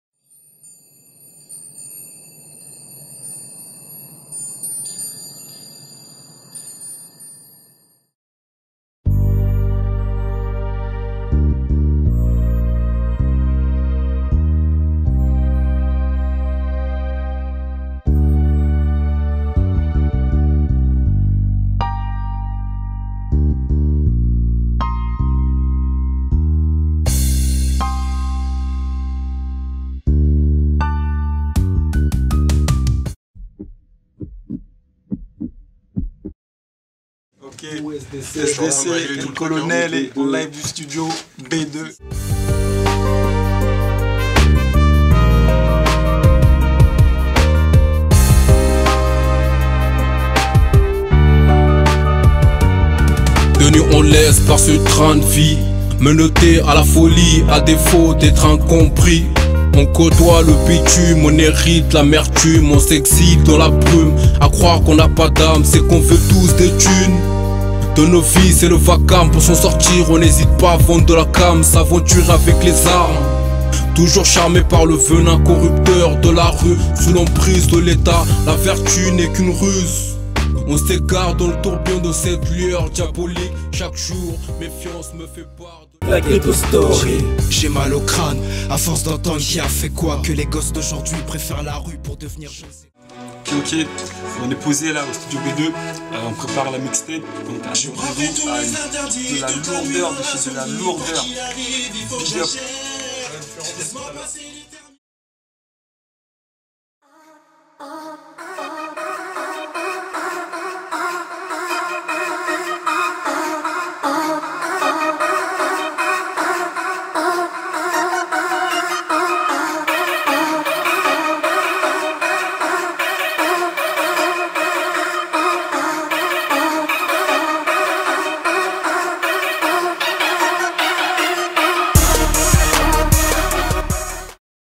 SDC (PAPOO&DIASTY) - au studio B2 .mp4
petit extrait du SDC en preparation de leur nouveau projet qui va arriver tres bientot restez connecter sa s'annonce tout simplement excellent pour suivre l'actu du SDC
et restez connecte aussi sur la page diamant vision pour les nouveaute